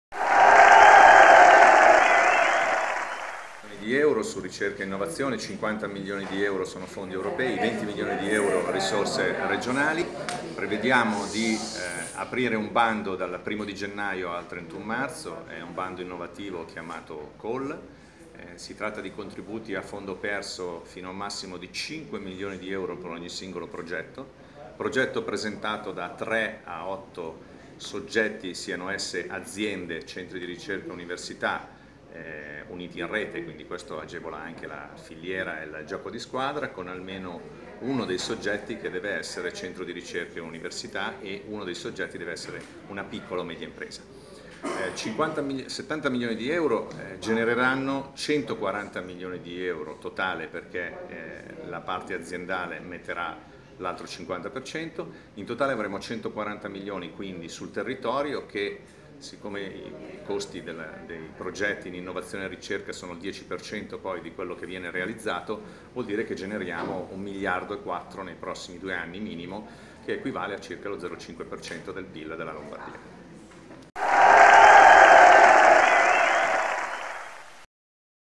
Regione Lombardia, vicepresidente Sala: "Al via bando da 70 milioni per la ricerca"
RISORSE DESTINATE A PROGETTI PRESENTATI DA IMPRESE E ATENEI "AUMENTIAMO NOSTRA COMPETITIVITÀ CON TRASFERIMENTO TECNOLOGICO"

Un bando da 70 milioni di euro destinato a Organismi di Ricerca, Università e Imprese aggregati in forma di partenariati, composti da un minimo di 3 soggetti a un massimo di 8, di cui almeno una Pmi e un Organismo di ricerca. Lo ha presentato il vicepresidente di Regione Lombardia e assessore alla Ricerca, Innovazione, Università, Export e Internazionalizzazione delle imprese Fabrizio Sala.

I CONTRIBUTI - I contributi, sia per attività di Ricerca industriale che di Sviluppo Sperimentale, risultano pari al 60 per cento delle spese ammesse per le Piccole Imprese, al 50 per le Medie e al 40 per le Grandi Imprese e Organismi di Ricerca. L'agevolazione concessa per singolo Partenariato non potrà essere superiore a 5 milioni di euro.

I SETTORI COINVOLTI - La 'call hub ricerca e innovazione' prevede il finanziamento di progetti di ricerca industriale e sviluppo sperimentale in una di queste 7 Aree di Specializzazione o nel driver trasversale di sviluppo smart cities and communities: Aerospazio; Agroalimentare; Eco-industria; Industrie creative e culturali; Industria della Salute;

Manifatturiero Avanzato; Mobilità sostenibile.

LE RISORSE GENERATE - "Stimiamo di generare 140 milioni di euro su progetti di ricerca e innovazione - ha commentato il vicepresidente Sala -, visto che le imprese coprono i costi al 50 per cento. I progetti muoveranno investimenti pari a 1,4 miliardi, l'equivalente dello 0,5 per cento del Pil della Lombardia".

BANDO INNOVATIVO, CHE PREMIA LE ECCELLENZE - "E' un bando innovativo, che aprirà a gennaio - ha detto ancora Sala - e si chiuderà a fine marzo, poi andrà in negoziazione. Il termine massimo per l'avvio dei progetti è a 30 mesi". "Premiamo le eccellenze, le idee che abbiamo nelle università, nei Centri di ricerca e nelle aziende - ha concluso il vicepresidente - in linea con la nostra impostazione che vede in primo piano il trasferimento tecnologico".

FACILITARE RELAZIONI - I principali obiettivi strategici della misura sono tre. In primo luogo, facilitare le relazioni tra Università, imprese, centri di ricerca e cittadini e la messa a sistema delle competenze strategiche e delle eccellenze presenti sul territorio, al fine di favorire il trasferimento tecnologico e l'utilizzo di nuove tecnologie a sostegno del processo di innovazione.

PROMUOVERE HUB TERRITORIALI - Ulteriore finalità è promuovere hub territoriali della conoscenza, della ricerca e dell'innovazione con proiezione internazionale - intesi come forme evolute di partenariato che, da un lato, integrino i tre elementi del triangolo della conoscenza (ricerca, innovazione, istruzione) e, dall'altra, prevedano forme di coinvolgimento degli utilizzatori finali (living lab) -, favorendo il loro rafforzamento e la loro partecipazione alle iniziative internazionali, anche nell'ottica di reciproche collaborazioni progettuali.

PROMOZIONE SVILUPPO DURATURO E SOSTENIBILE E CRESCITA - L'altro elemento strategico consiste nella promozione dello sviluppo duraturo e sostenibile e della crescita competitiva, attraverso il potenziamento delle dotazioni materiali ed infrastrutturali della ricerca e innovazione per la maturazione tecnologica e il trasferimento tecnologico e delle conoscenze.